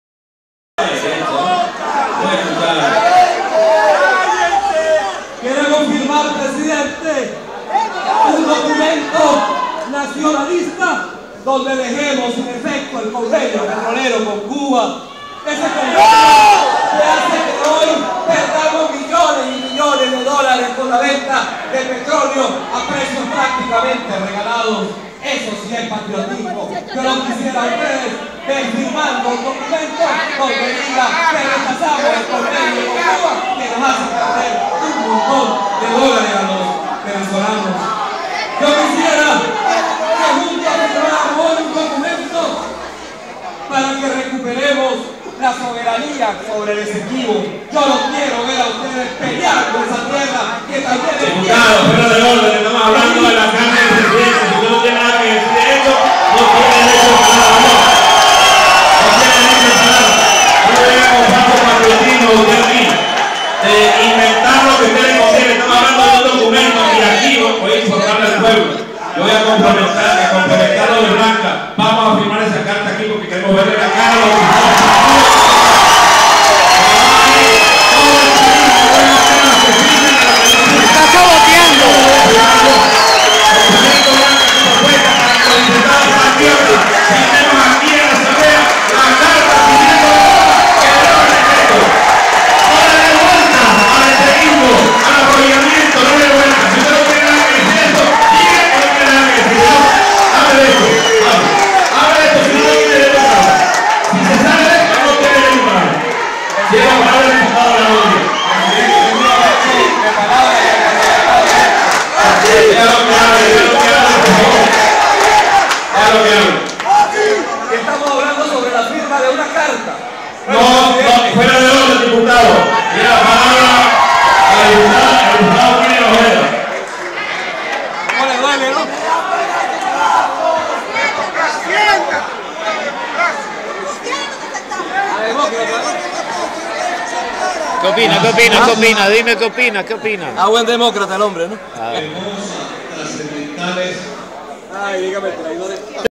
(video inédito) Cabello le quita el derecho de palabra a Abelardo Díaz
ND / 26 mar 2015.- El diputado de la Movida Parlamentaria, Luis Barragán, grabó el momento en que el presidente de la Asamblea Nacional, Diosdado Cabello,vinterrumpió al diputado Abelardo Díaz y luego le quitó el derecho de palabra.

Esto ocurrió durante la sesión del pasado martes en la que se discutió el apoyo o no a la posición del oficialismo sobre el decreto de Barack Obama.

Allí, Diaz aseguró que estaría de acuerdo con firmar un documento que "deje sin efecto el convenio petrolero con Cuba” o se refiera a la recuperación de “la soberanía sobre el Esequibo”.

"Yo los quiero ver a ustedes pelear por esa tierra..." dijo antes de que Cabello lo interrumpiera: "Diputado, fuera de orden..."

Durante la intervención se oyen gritos desde el palco en su contra.